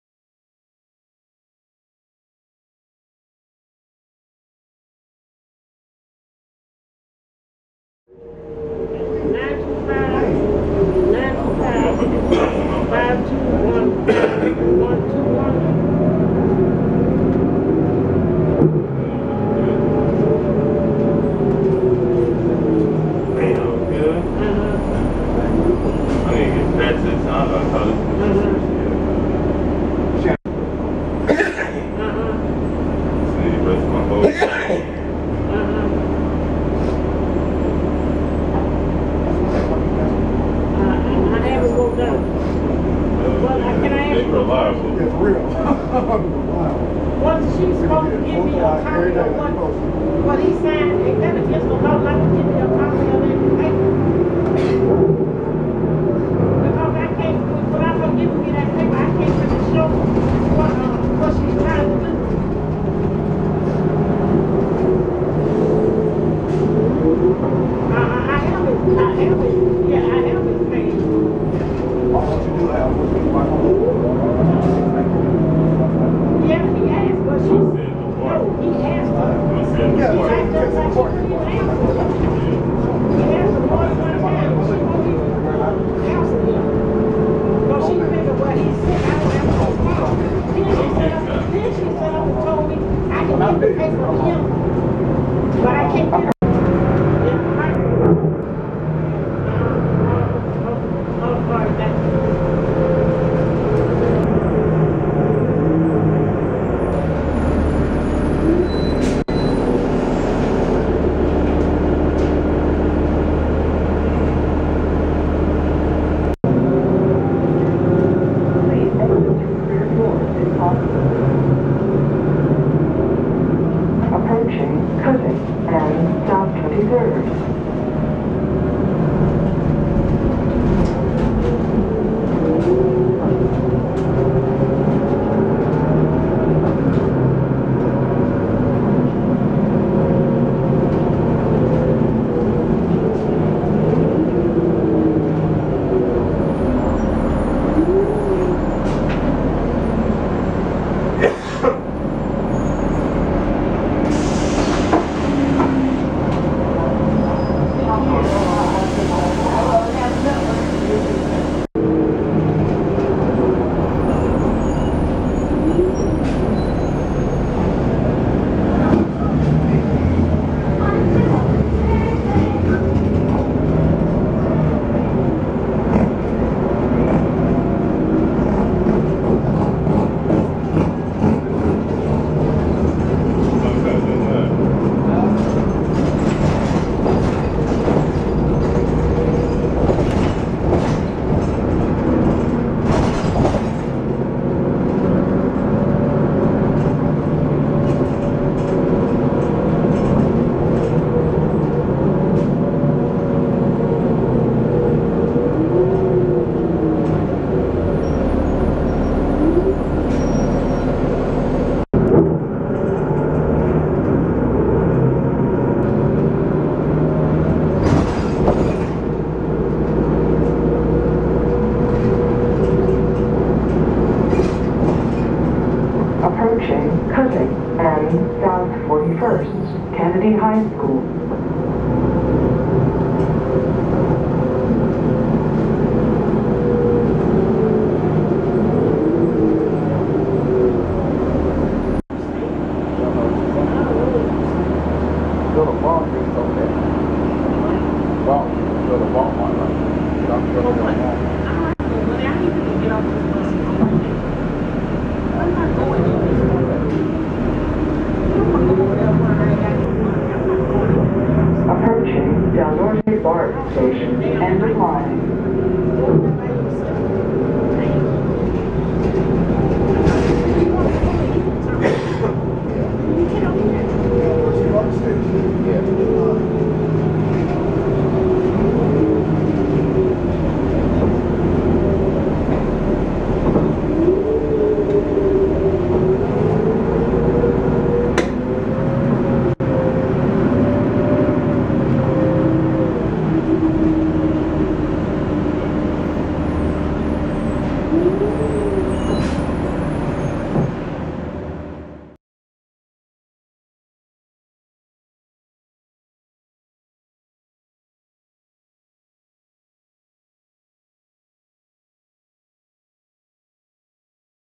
AC Transit 2013 Gillig Advantage 40' - 1350
This is a ride aboard a 2013-built Gillig Advantage (with the standard front end). AC Transit took delivery of 63 of these buses, with another one delivered from 2012.

1350 is seen here on a route 76, from Richmond BART station to the El Cerrito Del Norte transit center and BART station, on January 22nd, 2014.